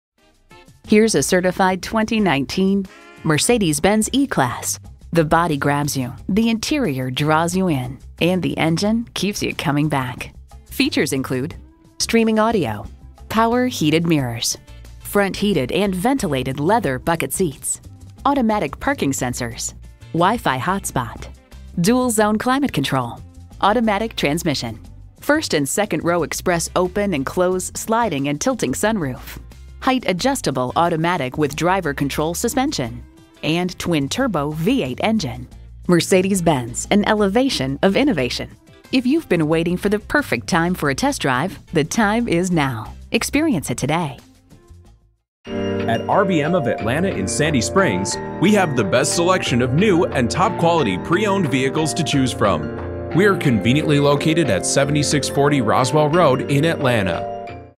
Certified 2019 Mercedes-Benz E-Class Atlanta GA Sandy Springs, GA U15540 - SOLD
Year: 2019
Make: Mercedes-Benz
Model: E-Class
Trim: AMG E 63 S 4MATIC+ Sedan
Engine: 8 Cyl. 4.0 L
Transmission: Automatic
Color: designo Cardinal Red Metallic
Interior: Black
Mileage: 3404
Stock #: U15540
VIN: WDDZF8KB1KA570479
CERTIFIED, ONE OWNER, CLEAN CARFAX! WHEELS: 20 AMG FORGED CROSS-SPOKE BLACK, WHEEL LOCKING BOLTS, SUN PROTECTION PACKAGE -inc: Rear Window Manual Roller Blinds, Power Rear-Window Sunshade, SOFT-CLOSE DOORS, RAPID HEATING FRONT SEATS, PANORAMA ROOF, OMISSION OF RED OR SILVER SEATBELTS, METAL WEAVE TRIM, EXTERIOR LIGHTING PACKAGE -inc: LED Intelligent Light System, Adaptive Highbeam Assist, DRIVER ASSISTANCE PACKAGE -inc: Active Emergency Stop Assist, Evasive Steering Assist, Active Blind Spot Assist and Active Lane Keeping Assist, PRE-SAFE PLUS Rear End Collision Protection, Active Distance Assist DISTRONIC, Active Steering Assist, Active Brake Assist w/Cross-Traffic Function, Active Speed Limit Assist, Extended Restart by Stop & Go Traffic, Active Lane Change Assist, PRESAFE Impulse Side, Route Based Speed Adaptation. This Mercedes-Benz E-Class has a powerful Twin Turbo Premium Unleaded V-8 4.0 L/243 engine powering this Automatic transmission.*Experience a Fully-Loaded Mercedes-Benz E-Class AMG E 63 S 4MATIC+ Sedan *AMG NIGHT PACKAGE -inc: AMG Exterior Night Styling, high-gloss black front splitter, high-gloss black mirror housings, high-gloss black side fender inserts, high-gloss black window frame trim, high-gloss black rear apron trim strip and black chrome tailpipe, ACTIVE FRONT SEATS PACKAGE W/MASSAGE FEATURE, ACOUSTIC COMFORT PACKAGE -inc: increased cabin insulation and windshield and side windows w/acoustic and heat absorbing membrane, Infrared-Reflecting Glass , AMG PERFORMANCE EXHAUST SYSTEM, AIR BALANCE CABIN AIR FRAGRANCE & FILTRATION SYS, Window Grid Antenna, Wheels: 20 AMG Twin 5-Spoke w/Grey Accents, Valet Function, Trunk/Hatch Auto-Latch, Trip computer, Transmission: AMG SPEEDSHIFT MCT 9-Speed, Transmission w/Driver Selectable Mode, Tracker System, Tires: 265/35ZR20 Front & 295/30ZR20 Rear -inc: High-performance summer, Tire Specific Low Tire Pressure Warning, Systems Monitor, Surround View Back-Up Camera, Steering Wheel Shift Paddles, Spare Tire Mobility Kit.*Stop By Today *Come in for a quick visit at RBM of Atlanta, 7640 Roswell Road, Atlanta, GA 30350 to claim your 2019 Mercedes-Benz E-Class AMG E 63 S 4MATIC+ Sedan!

Address:
7640 Roswell Road